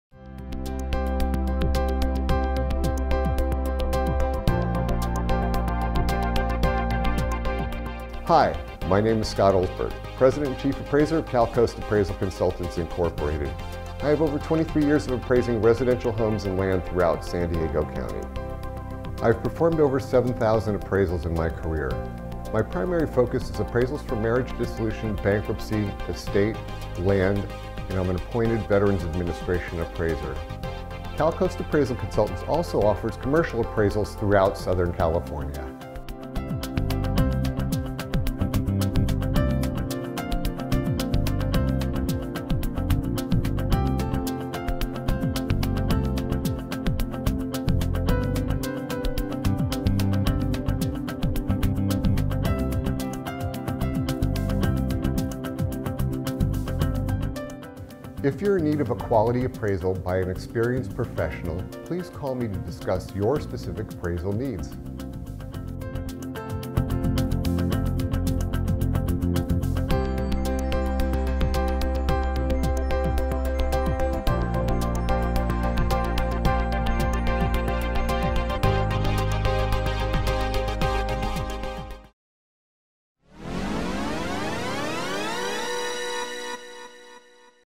San Diego Appraiser Scott Olsberg
San Diego appraiser Scott Olsberg covers San Diego County for residential appraisal and Southern California for commercial appraisal services.